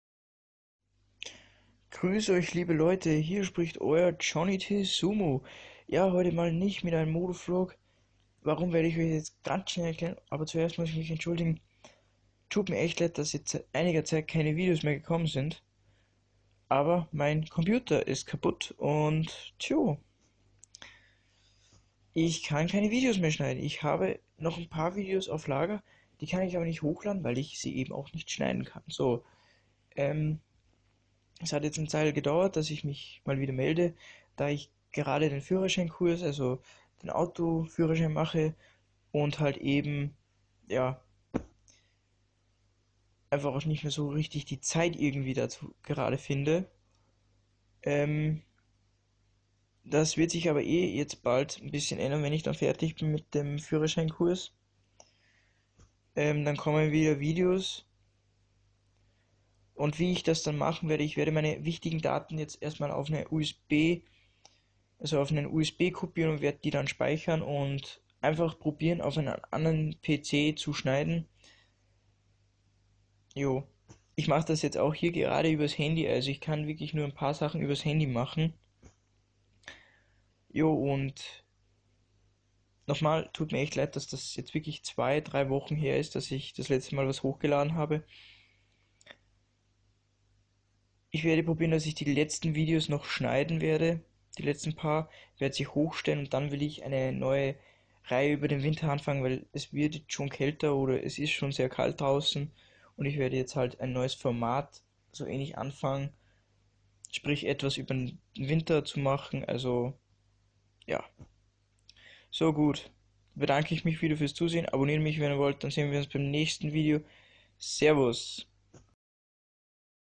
[JT] Warum kamen keine Videos?
Nochmals sorry das jetzt so lange keine Videos mehr gekommen sind!

Ich werde versuchen so schnell wir möglich wieder zu schneiden anzufangen, bis dahin habt geduld!

Ich werde eventuell​ öfters mal ein paar Updates machen wie es um PC und co. steht!

Euer Johnny